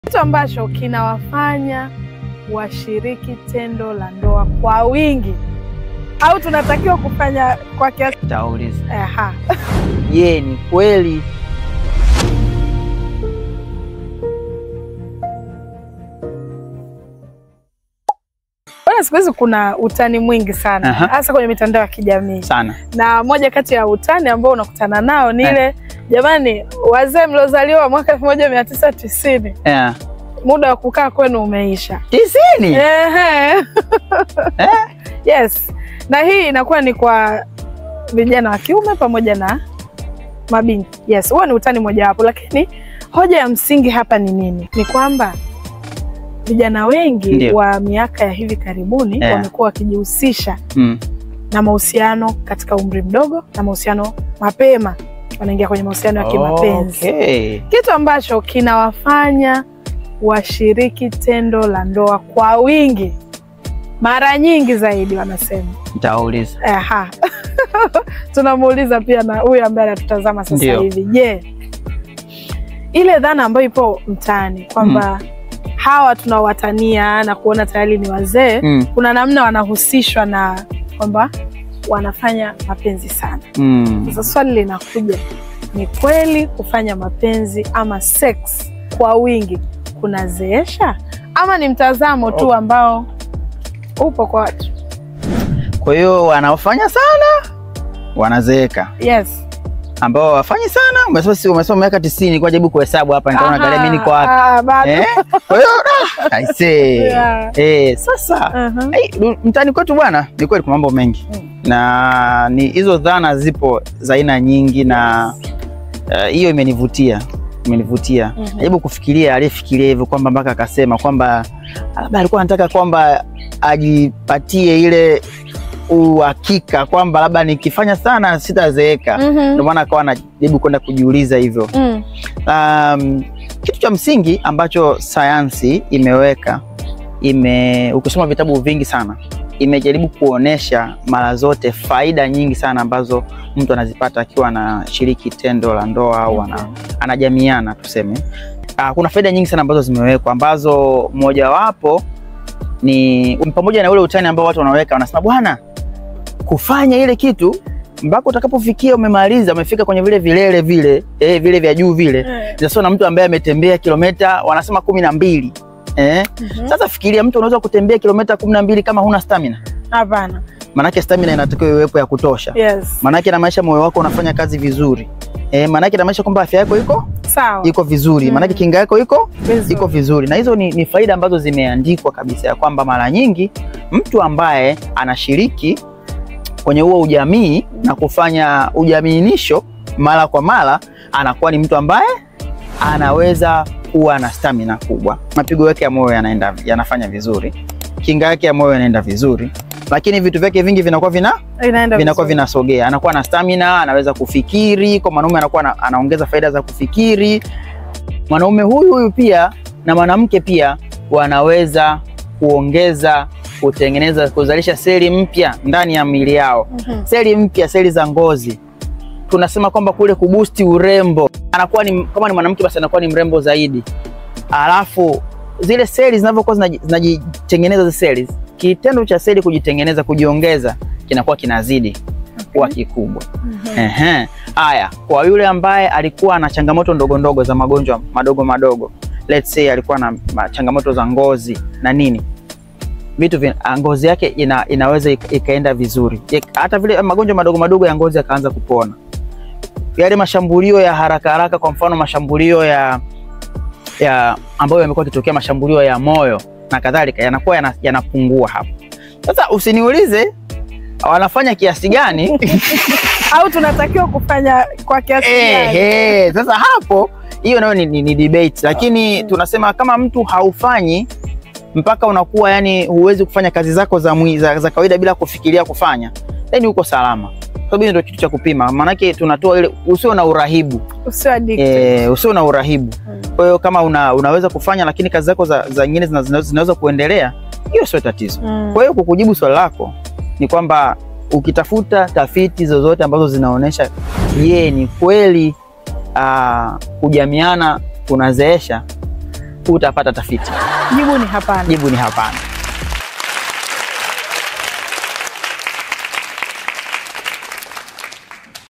Daktari Kijanjani: Je sex inazeesha?
Swali kutoka kwako, Je kufanya sex kwa wingi huzeesha... wanaofanya sana wanaokena wazee.
sawa majibu haya hapa.

mawasiliano;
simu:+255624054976

artwork:
Inessgrace: